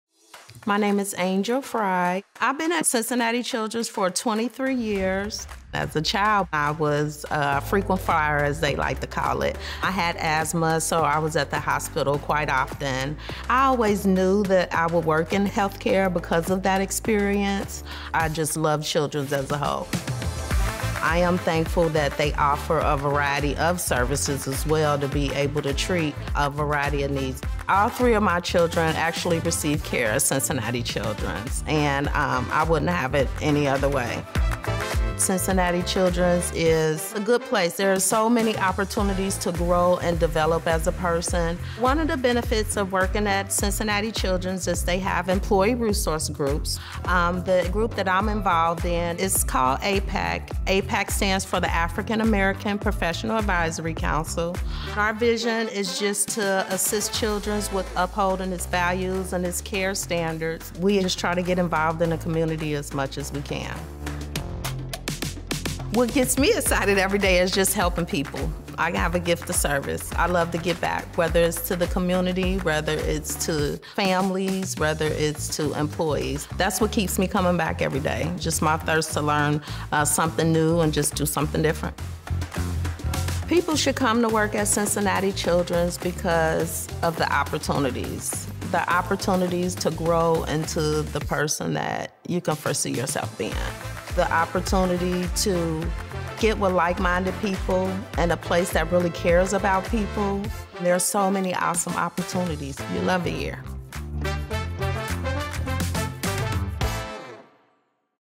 Employee Story: Angel Frye | Cincinnati Children's
Operations Specialist Angel Frye shares why she loves working at Cincinnati Children's and why she has stayed for 23 years. As former patient herself, she appreciates the supportive culture that enables growth and development, and the ability to give back though her  involvement with our Employee Resource Groups.